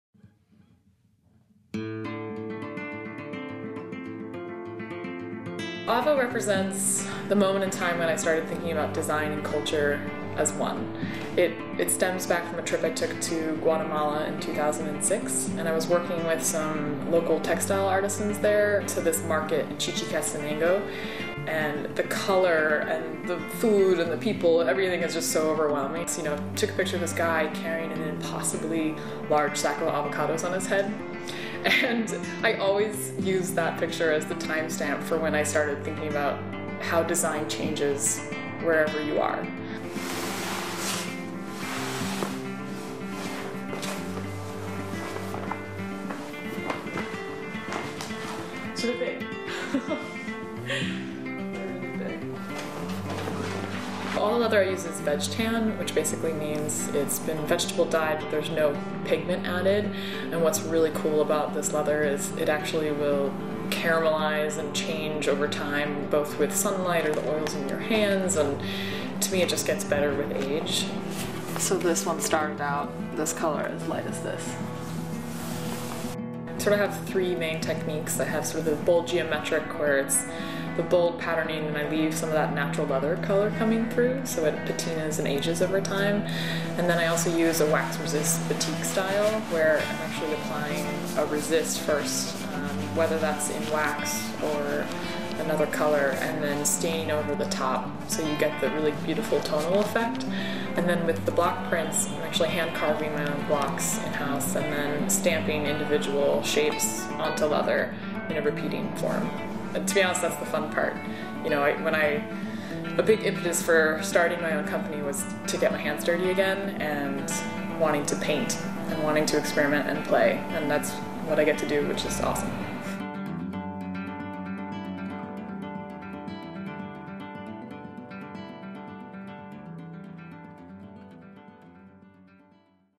Inside AVO Founder Brit Kleinman's Brooklyn Design Studio | Architectural Digest
For about a year, Brit Kleinman of young design label AVO would sit on the floor of her apartment in Brooklyn’s Fort Greene neighborhood, painting giant swaths of leather by hand. “When the hides were touching all four walls, I figured it was time to move to another space,” she says with a laugh, from the warehouse just down the street where she moved her practice. Here, the floor still serves as her primary workspace; bottles of dye line the desk, bins full of scraps are stacked along a wall, and folds of raw, vegetable-tanned leather cascade from a shelf.

The Rhode Island School of Design graduate had worked with leather for years, molding it into handbags and other accessories for brands like Shinola and Jack Spade, but in 2014, a bit of travel and an inherent sense of adventure nudged her to set out on her own. She’d become obsessed with parfleche, a type of painted hide envelope used by the Plains Indians in New Mexico. Inspired, she began experimenting with a similar technique, testing different paints, dyes, and leathers until she found just the right combination.

What began with a collection of painted leather rugs—soft leather hides painted with geometric patterns in nontoxic dyes—has expanded into pillows, and now wall tiles, a collection that she’s debuting at the Architectural Digest Home Design Show this week. The painted leather shapes can create bold motifs, resembling a tumbling-blocks-patterned wall treatment or inlaid stone, depending on your selection.